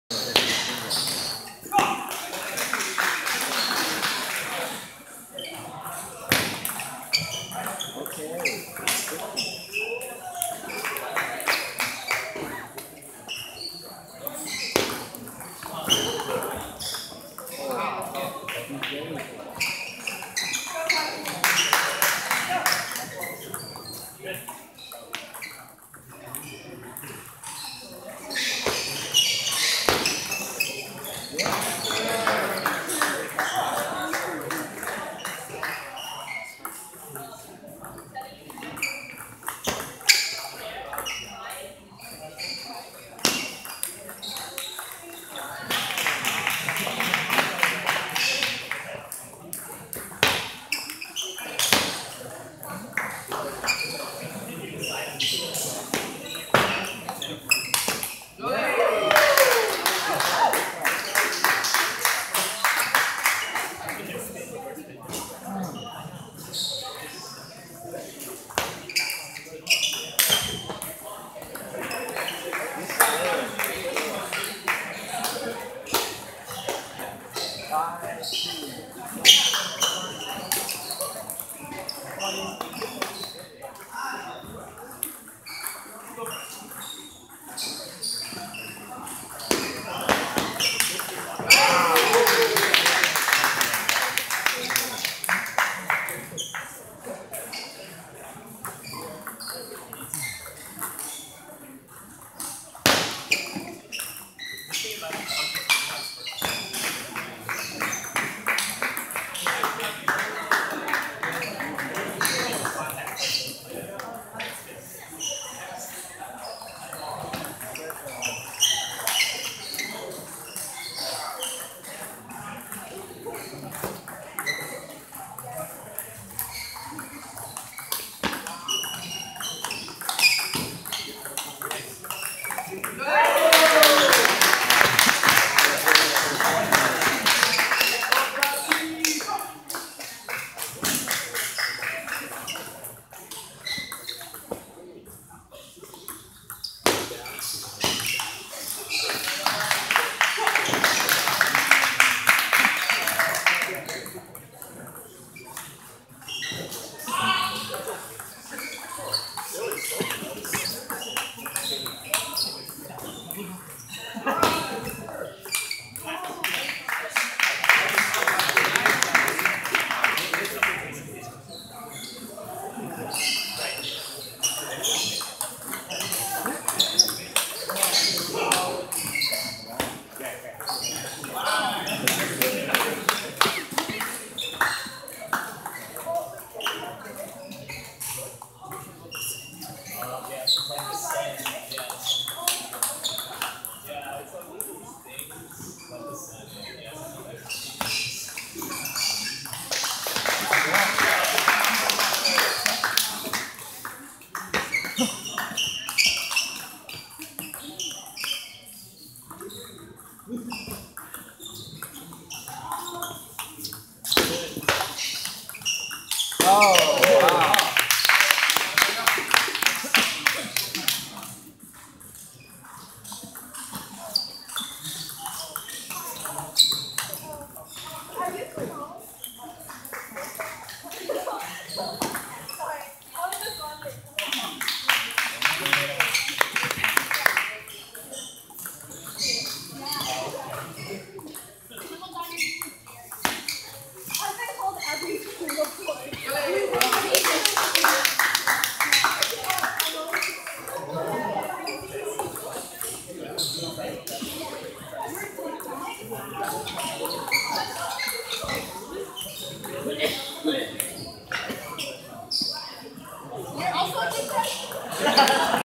Prachi UC Berkeley vs Emily Ohlone 20170304 121325
2017 NCTTA Western Regional at Topspin
Women's Single Semi-Final